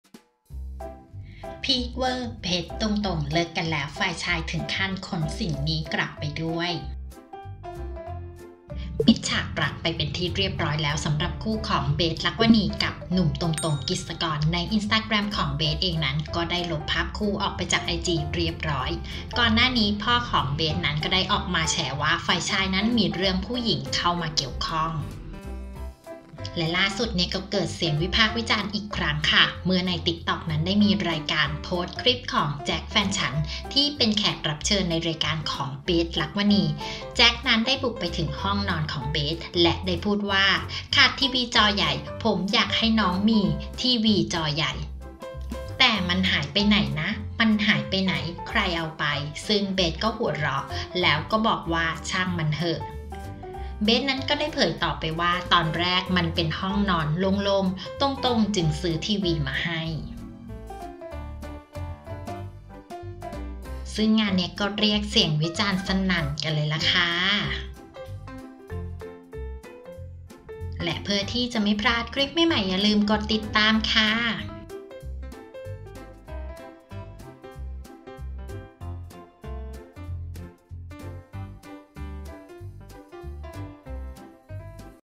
พีคเว่อร์! เบสท์-ตงตง เลิกกันเเล้ว ฝ่ายชายถึงขั้นขนสิ่งนี้กลับไปด้วย!!
ปิดฉากรักไปเเล้วสำหรับคู่ของ เบส รักษ์วนีย์ กับหนุ่ม ตงตง กฤษกร ในอินสตราแกรมของเบสท์เองก็ได้ลบภาพคู่ออกจากไอจีไปเเล้ว ก่อนหน้านี้พ่อของเบสท์ได้ออกมาเเฉว่าฝ่ายชายมีเรื่องผู้หญิงเข้ามาเกี่ยวข้อง

ล่าสุด ( 11 พ.ค. ) เกิดเสียงวิพากษ์วิจารณ์อีกครั้งเมื่อ ในติ๊กต็อกได้มีการโพสต์คลิปของ เเจ็ค เเฟนฉัน เป็นแขกรับเชิญในรายการของ เบสท์ รักษวนีย์ แจ็คได้ ไปถึงห้องนอนเบสท์ และได้พูดว่า ขาดทีวีจอใหญ่ ผมอยากให้น้องมีทีวีจอใหญ่ แจ่มันหายไปไหนนะ มันหายไปไหน ใครมันเอาไป ซึ่ง เบสท์ ก็หัวเราะ แล้วบอกว่า ชังมันเหอะ

เบสท์ได้เผยต่อว่า ตอนเเรกมันเป็นห้องนอนโล่งๆ ตงตง จึงซื้อทีวีมาให้